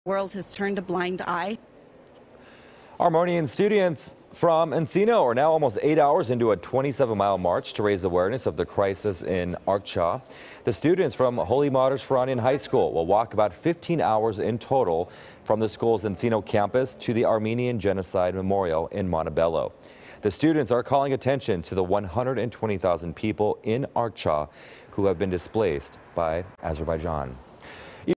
Encino high school students march for displaced Armenians
Students from the Holy Martyrs Ferrahian Armenian High School in Encino march to the Montebello Armenian Genocide Monument